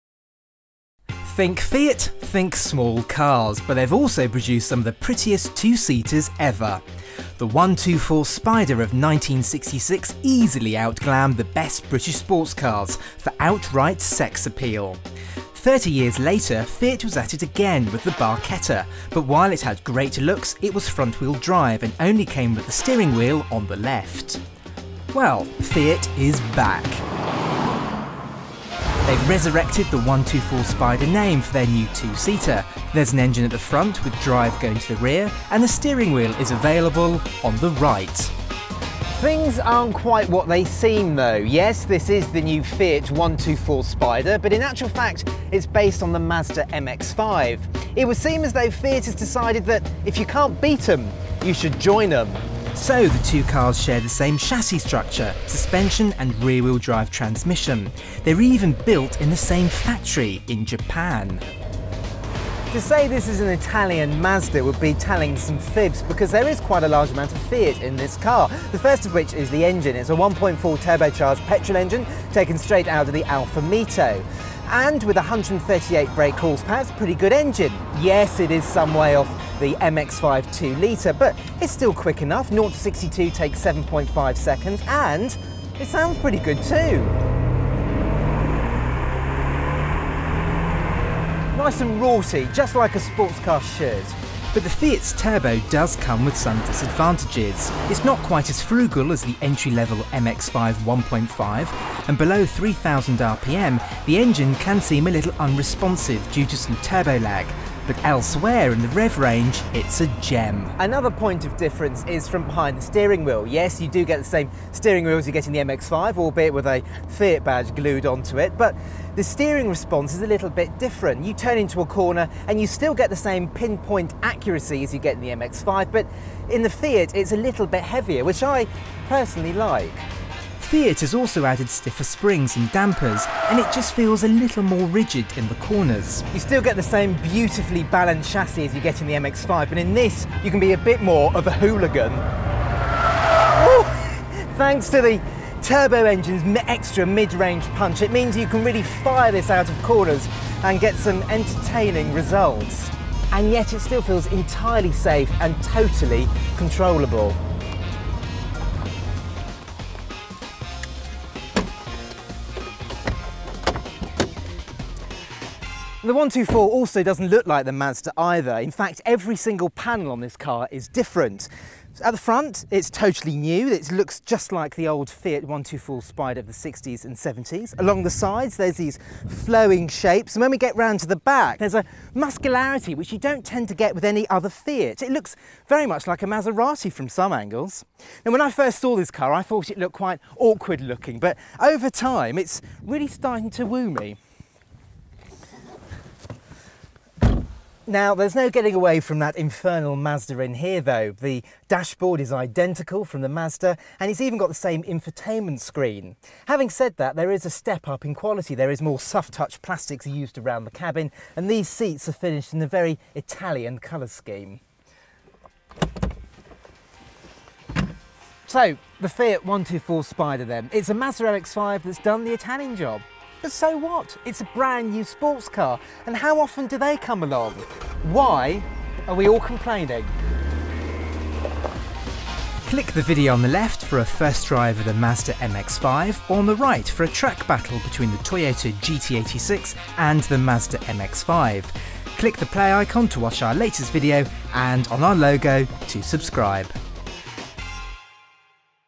Fiat 124 Spider review: Is it a true Italian sports car?
Fiat's Mazda MX-5 is here and we've taken to the roads in the 124 Spider. Has it taken a little help from Japan to resurrect the affordable Italian sports car? We just hat to find out...